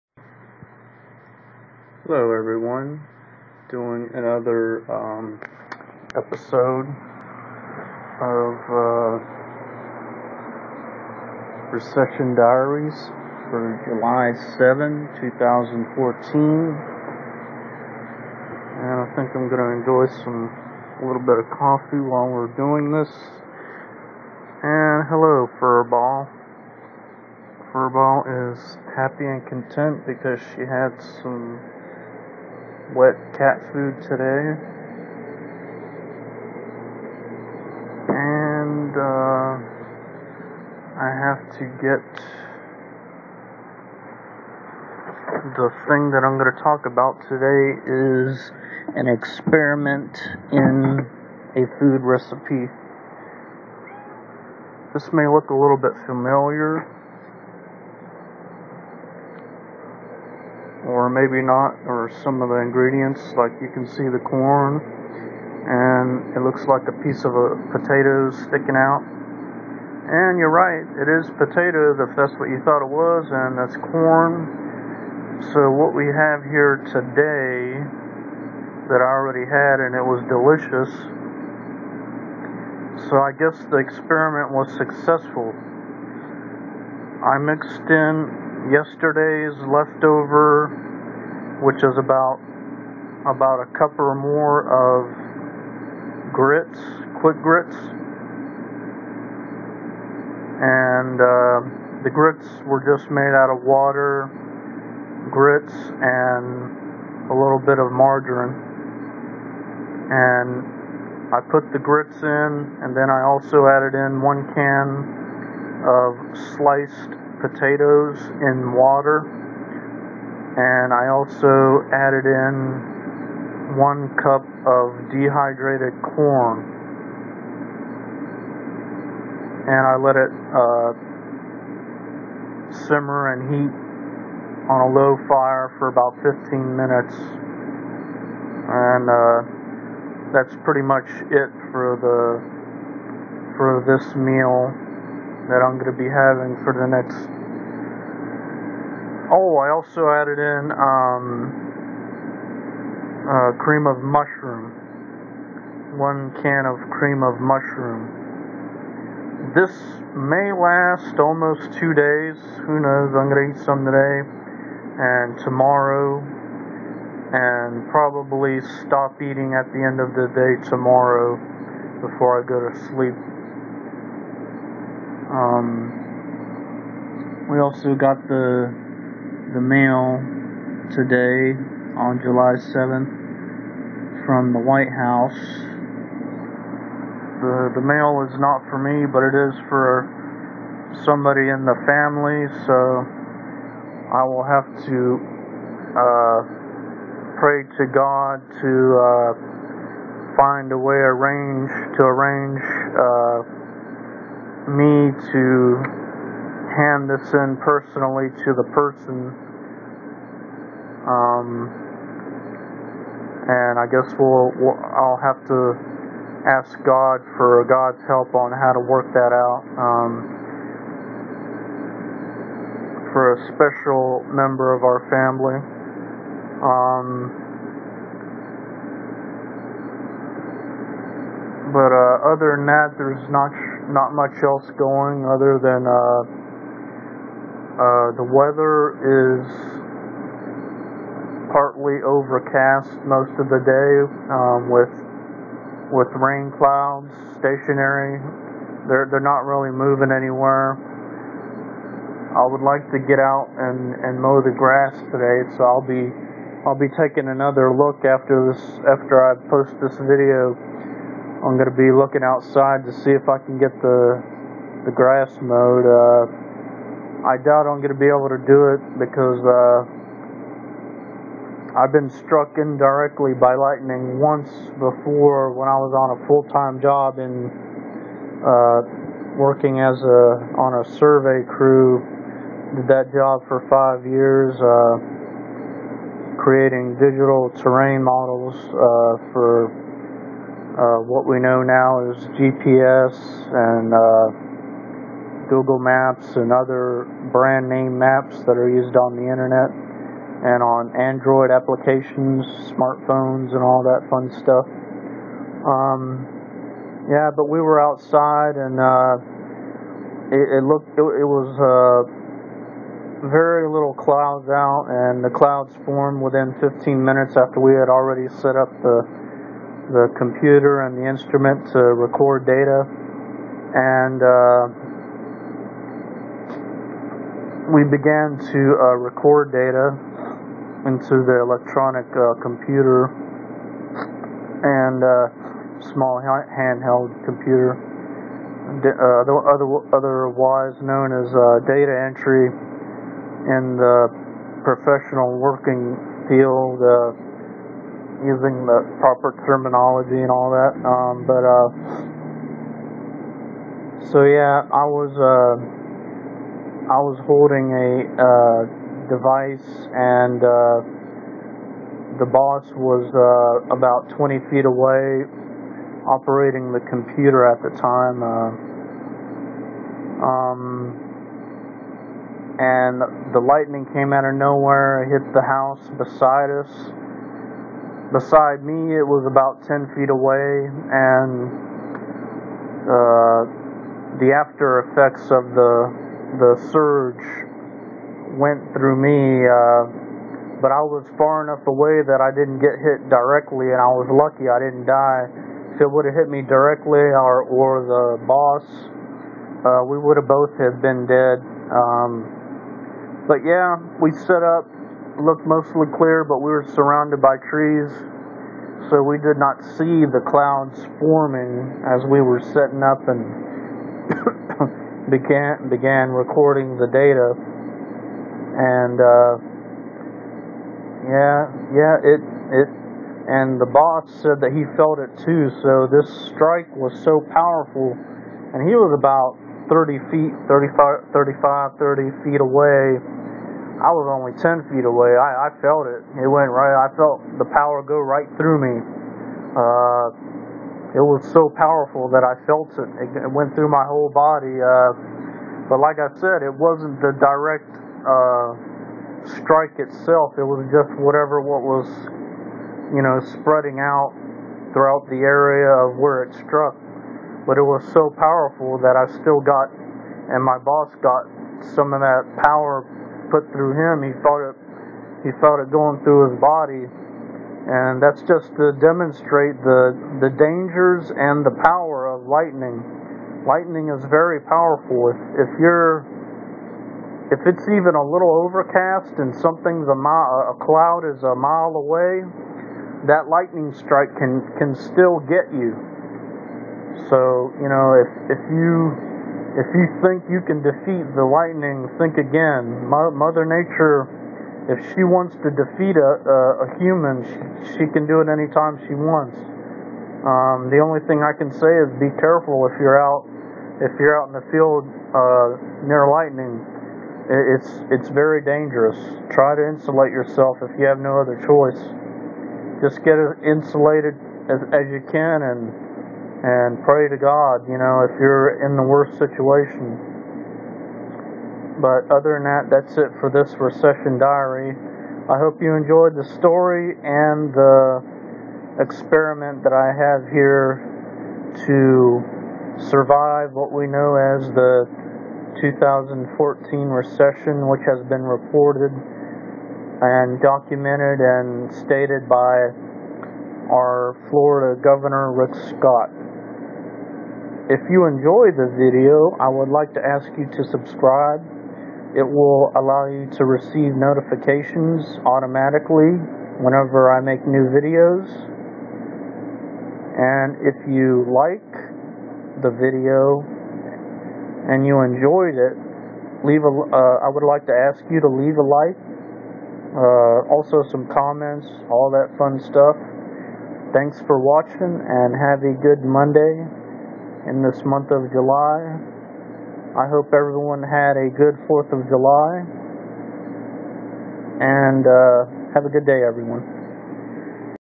Recession Diary July 7 2014
I experiment with food A and food B in order to conserve and extend nutrition resources during the recession of 2014.